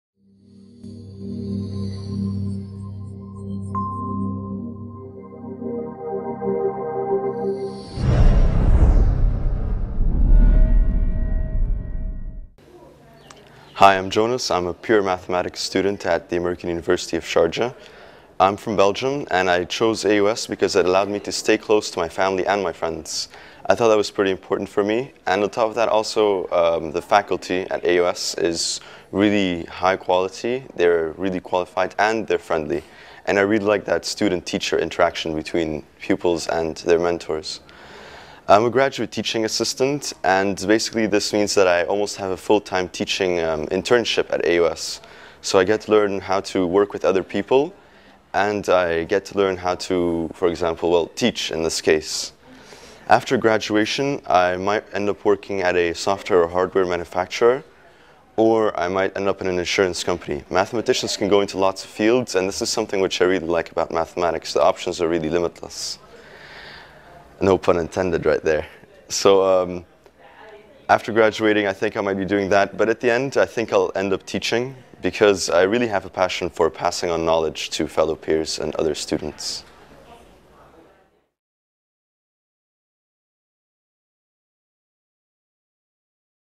AUSpeaks | Jonas Saman (MSc. in Mathematics)
Jonas Saman, American University of Sharjah (AUS) Master of Science in Mathematics program student from Belgium talks about what makes AUS a great place to pursue a graduate degree and the limitless options available for Math majors. He also talks about the quality of AUS faculty as well as his internship as a Graduate Teaching Assistant.

-For more information about graduate programs offered at the American University of Sharjah, please